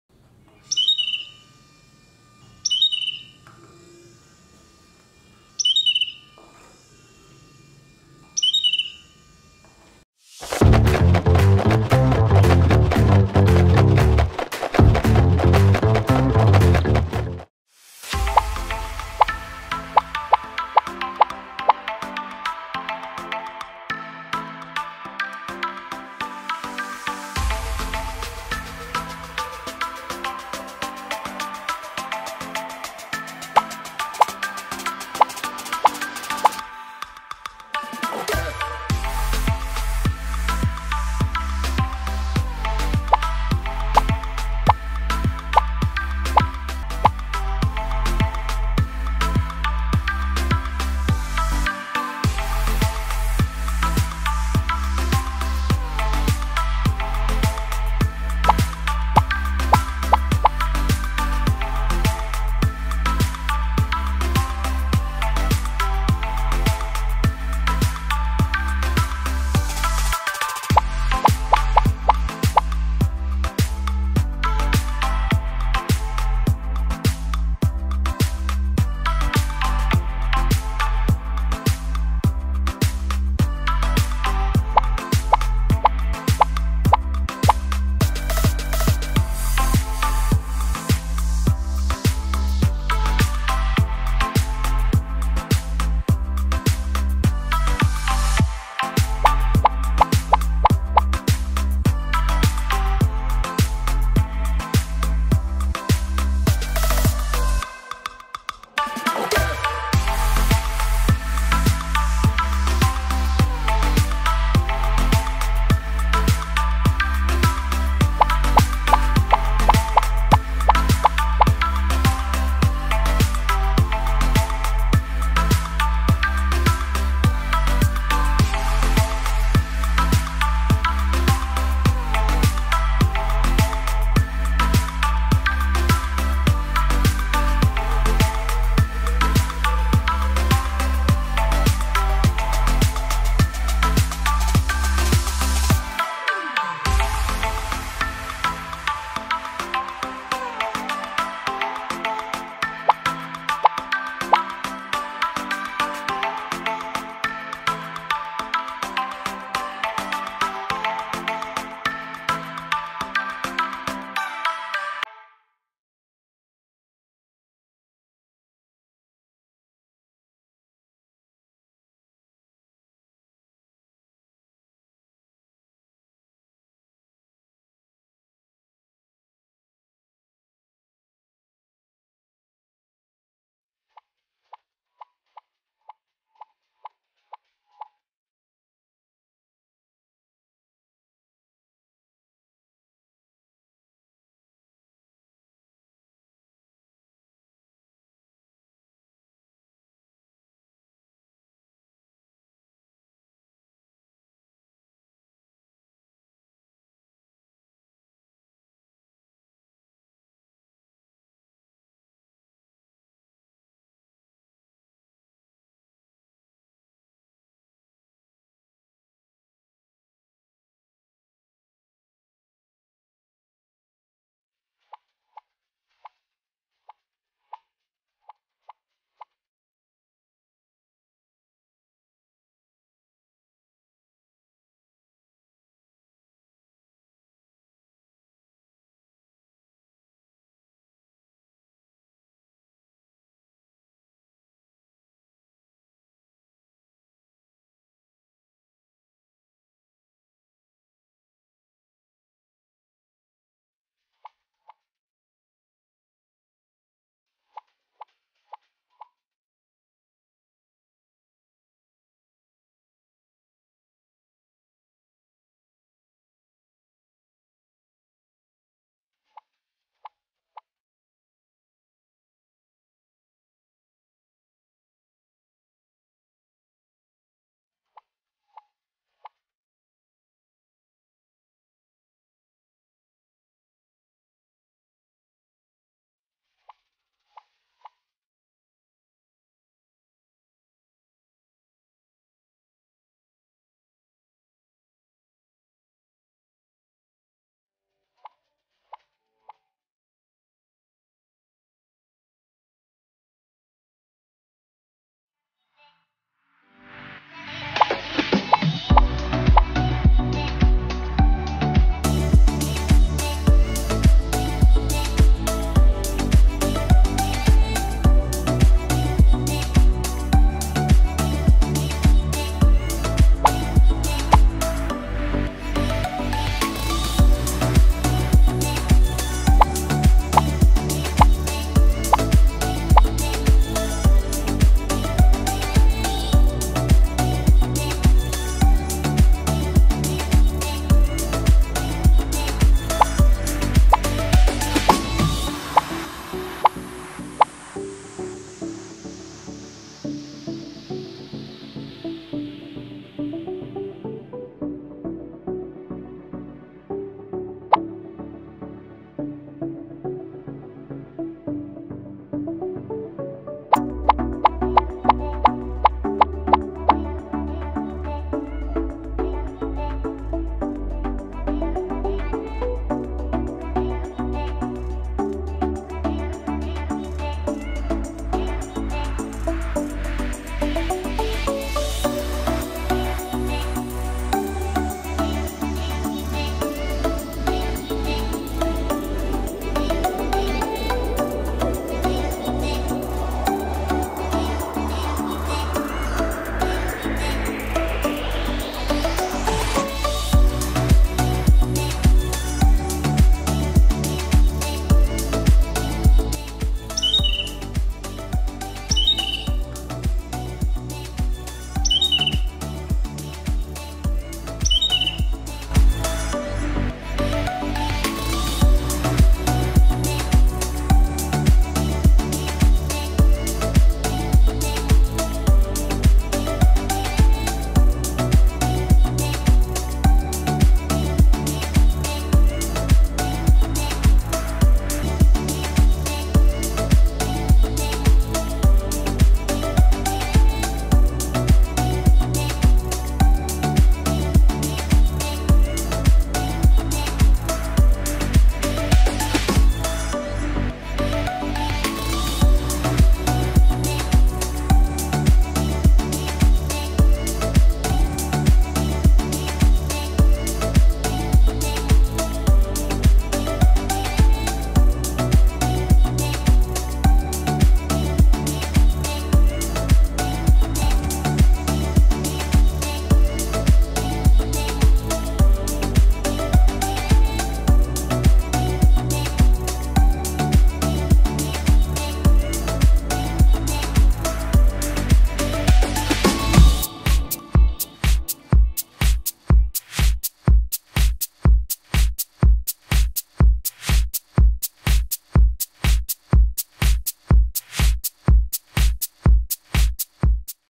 Lego Spike Essential Projects instructions + code Bulbul tweeted II LEGO EDUCATION
Looking for an engaging and interactive way to teach your kids about STEM? Introducing the Spike Essential Bird Sound Robot! In this video, we demonstrate how this innovative robot can help develop your child's passion for STEM in a fun and exciting way. Perfect for parents and educators looking to inspire the next generation of innovators.